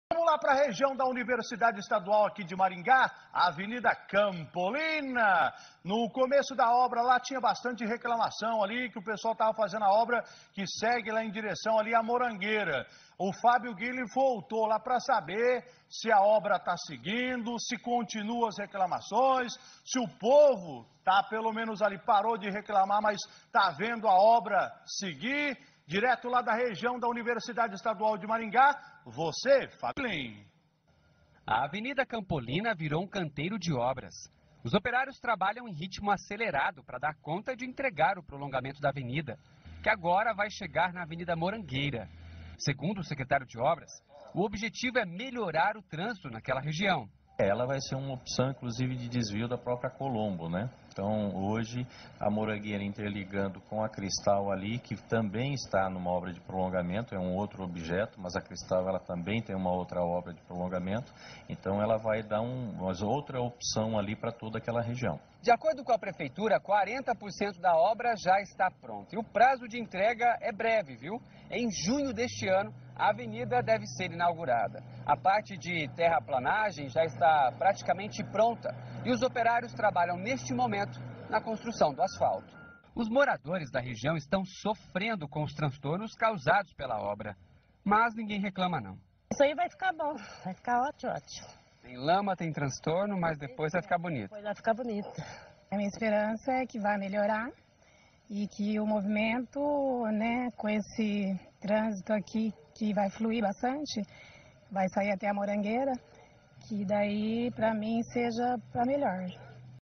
Obras na Avenida Campolina melhoram o tráfego
A avenida será aberta até a Morangueira. Isso vai ajudar no fluxo de veículos de outras ruas também. A obra está 40% pronta e o prazo para o término é até o meio do ano.